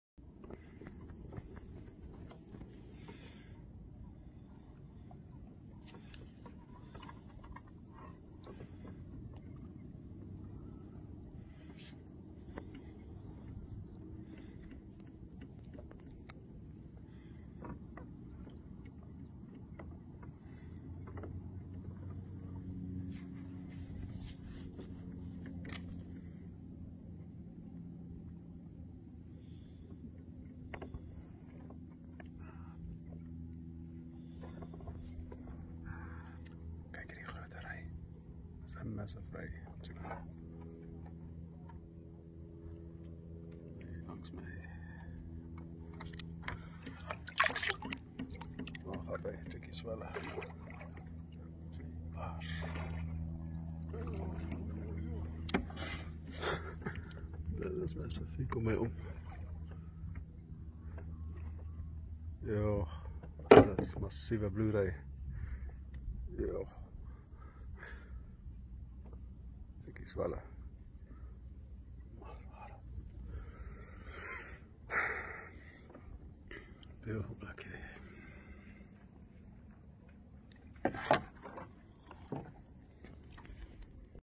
Good size Blue Ray (Dasyatis chrysonota) Knysna Mud Banks
Exploring and Fishing early Saturday morning, came across this beautiful Blue Ray. Knysna